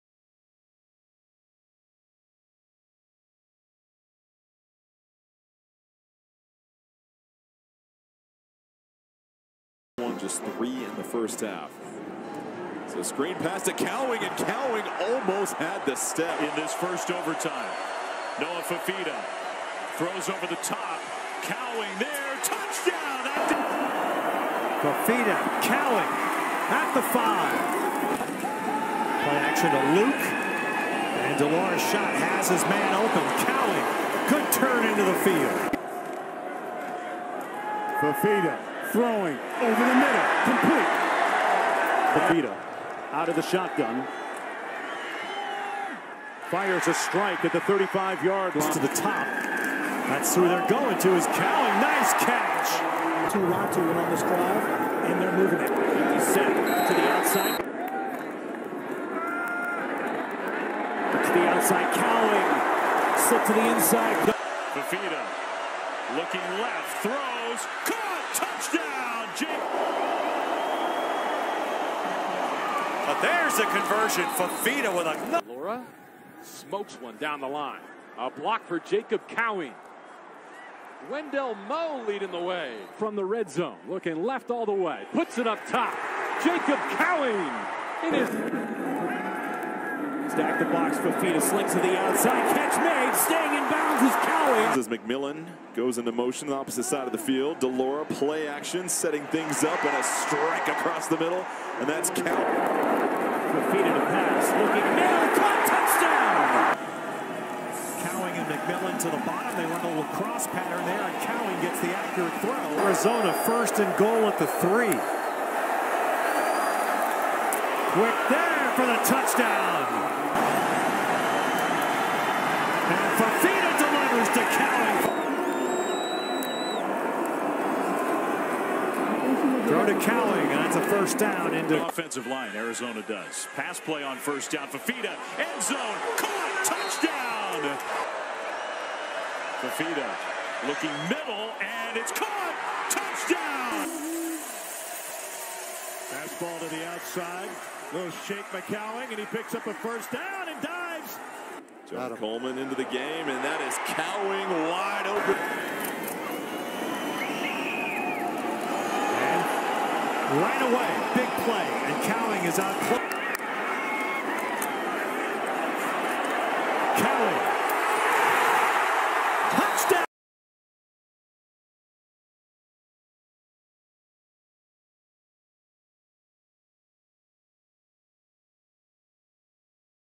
Jacob Cowing | 𝟚 | Arizona Wildcats WR
ARIZONA WILDCATS || JACOB COWING HIGHLIGHTS
58 CAREER GAMES (NCAA)
316 RECEPTIONS
4,477 YARDS
33 TOUCHDOWNS